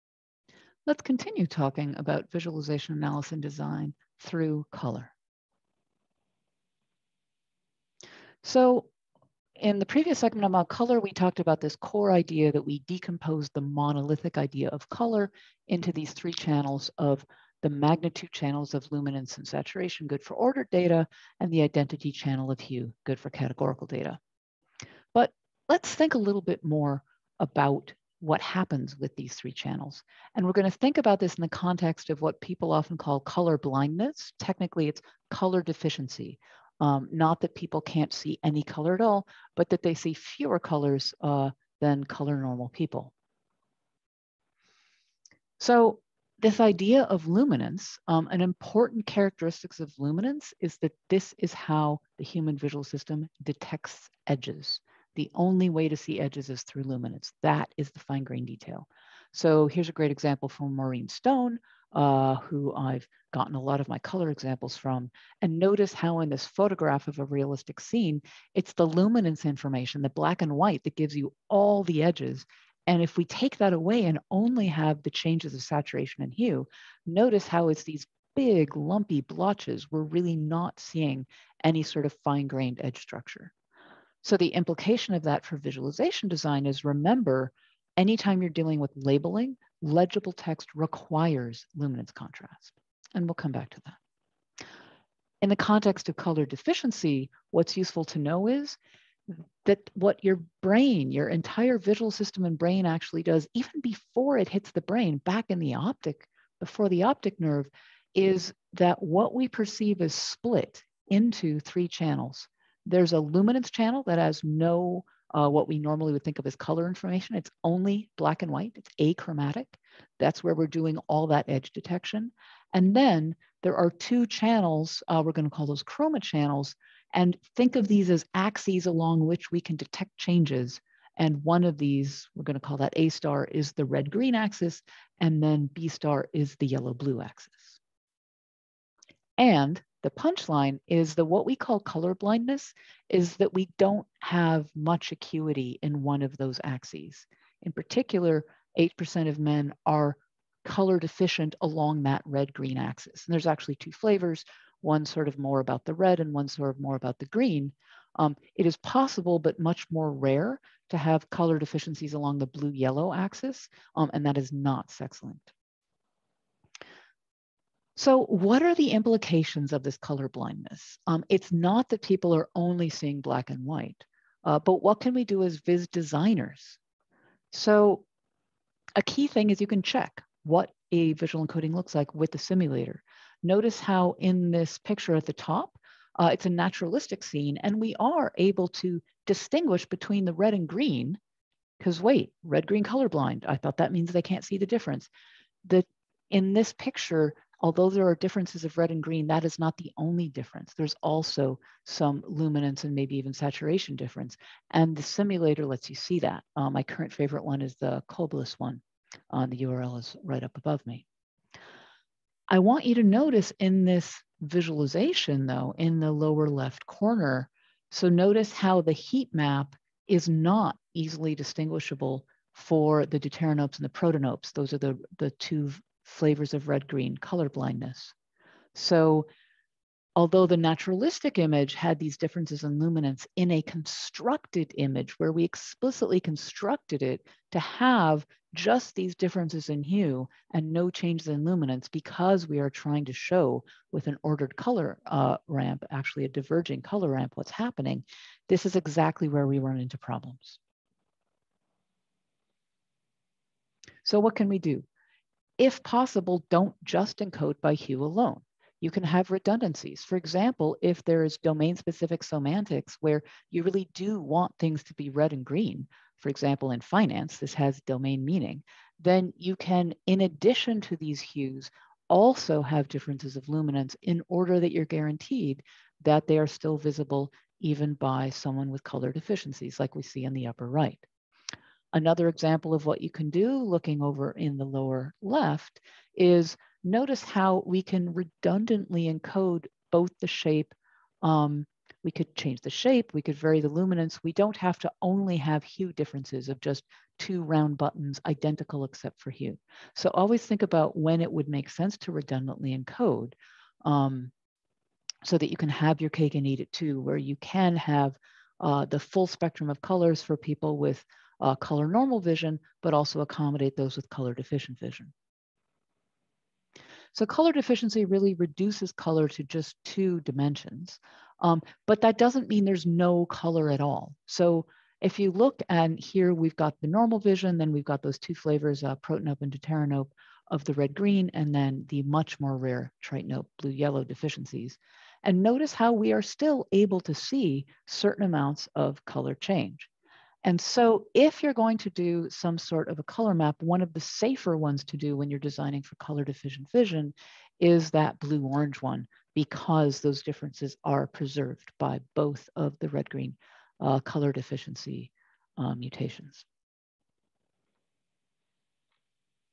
Color (Ch 10) II, Visualization Analysis & Design, 2021
Color II Lecture, 2021.
Map Color and Other Channels (Ch 10), Visualization Analysis & Design by Tamara Munzner, CRC/Routledge 2014.